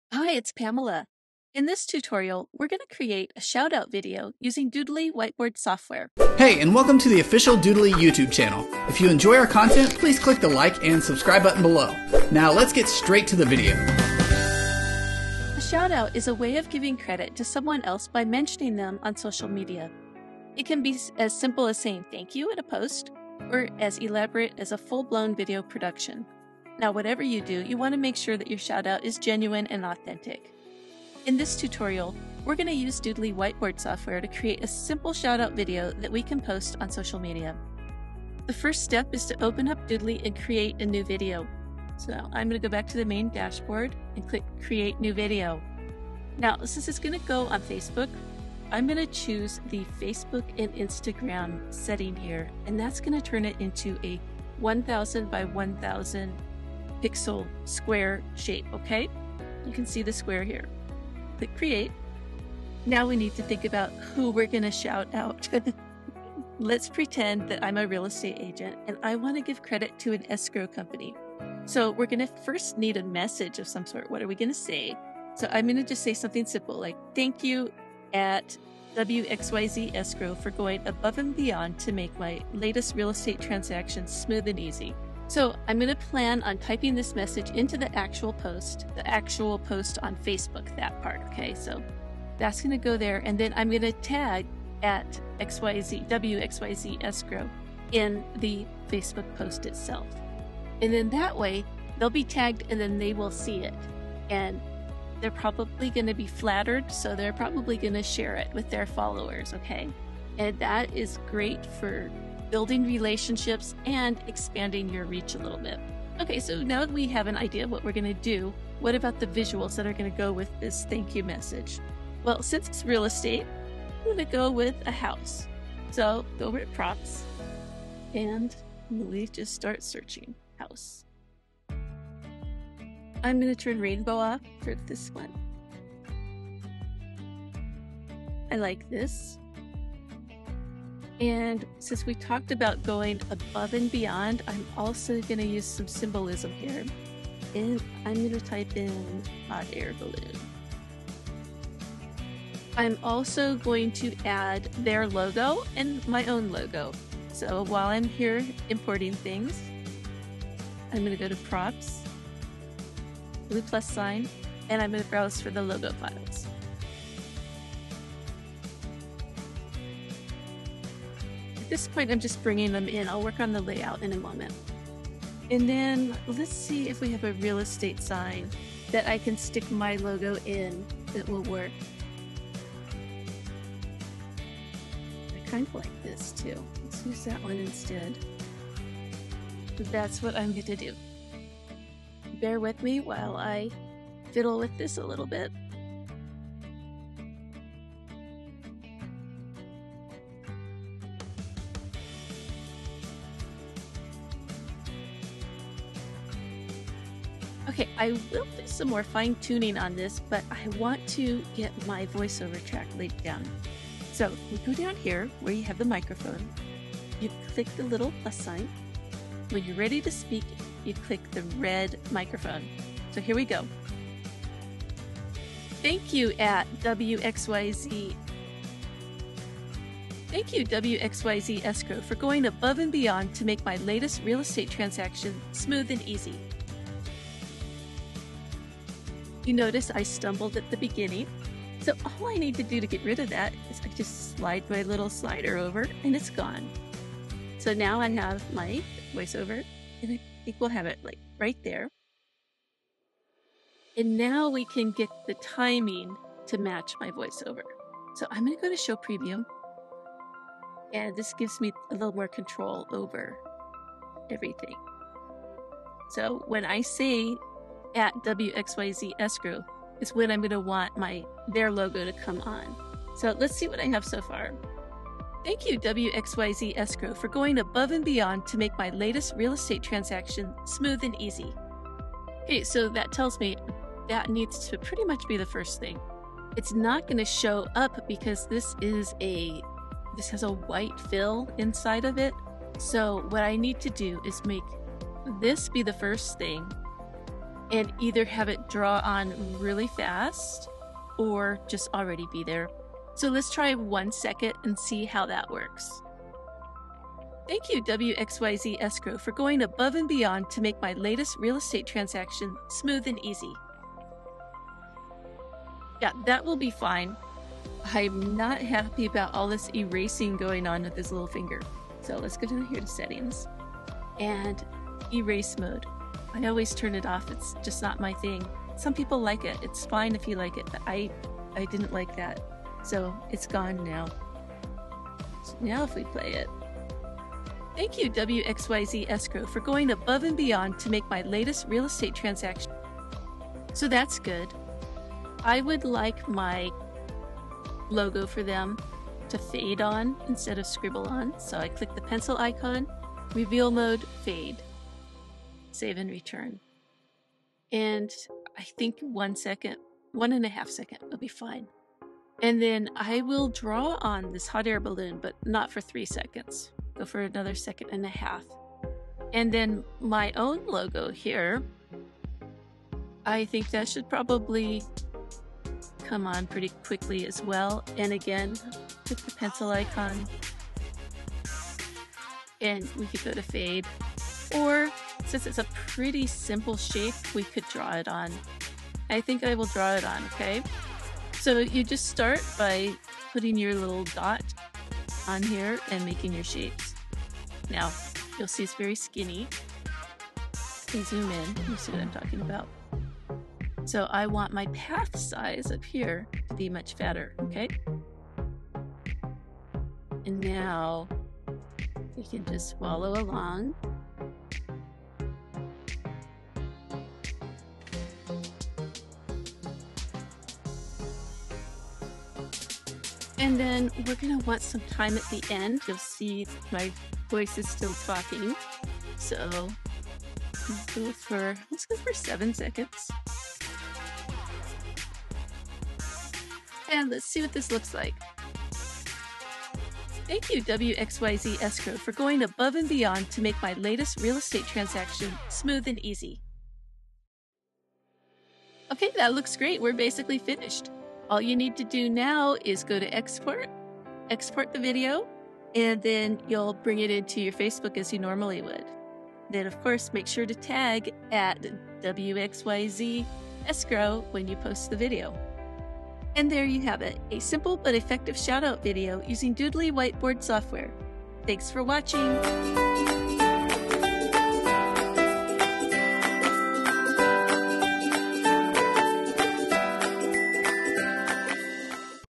Learn How to Create a Shoutout Video in Doodly
In today's tutorial, you’ll learn how to create a shoutout video with Doodly whiteboard software! There are so many benefits to making a shoutout this way. Pamela will show you how to use the square video format to create a simple and effective video that you can post on social media!